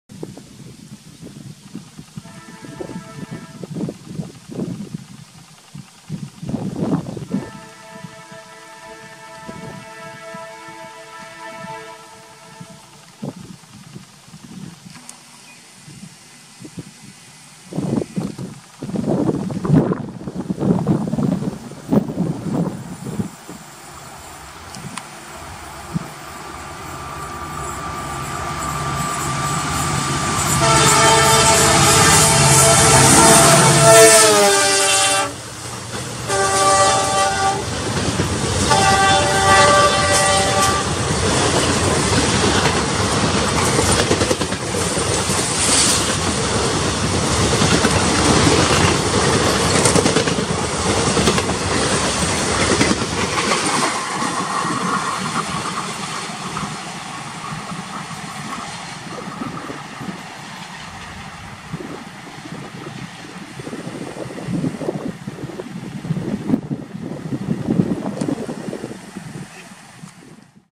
Furious action & honking 12630 KSK Exp
KJM WDP4D 40237 at the Masthihalli LC with the 12630 H Nizamuddin - YPR Jn. Karnataka Sampark Kranthi Express.

Incredible Indian Railways thru my lens
Masthihalli, Karnataka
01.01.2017